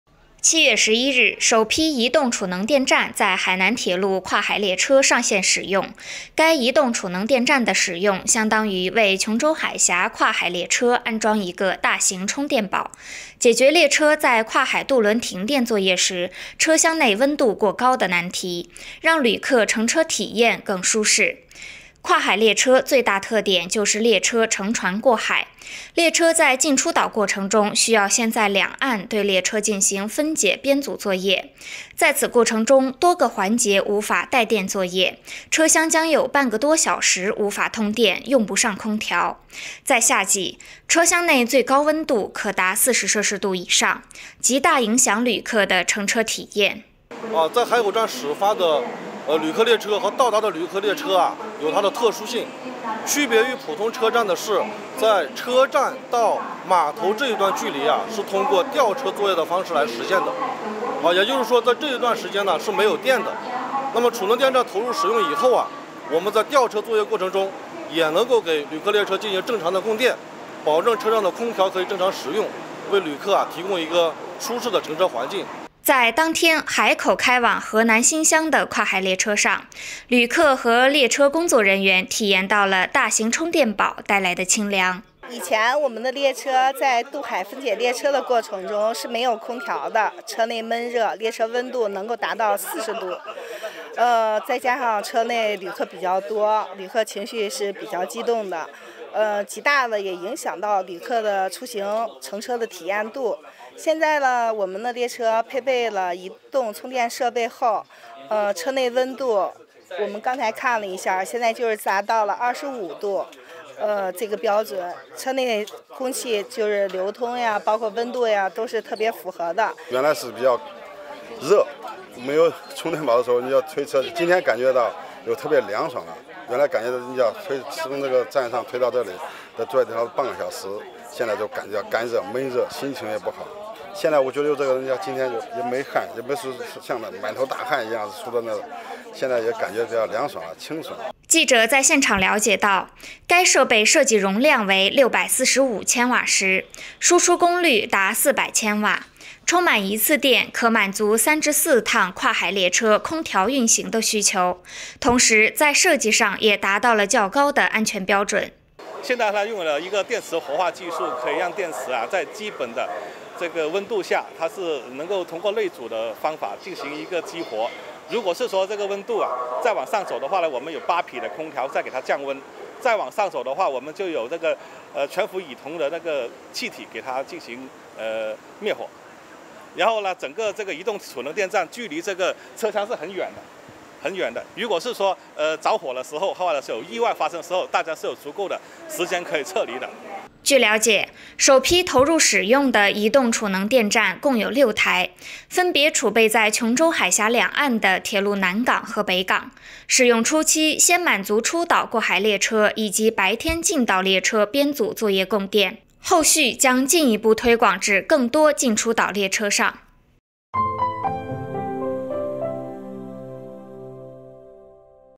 琼州海峡跨海列车安装“大型充电宝”为旅客送清凉
7月11日，首批移动储能电站在海南铁路跨海列车上线使用。该移动储能电站的使用，相当于为琼州海峡跨海列车安装一个“大型充电宝”，解决列车在跨海渡轮停电作业时车厢内温度过高的难题，让旅客乘车体验更舒适。

据了解，首批投入使用的移动储能电站共有6台，分别储备在琼州海峡两岸的铁路南港和北港。使用初期先满足出岛过海列车以及白天进岛列车编组作业供电。后续将进一步推广至更多进出岛列车上。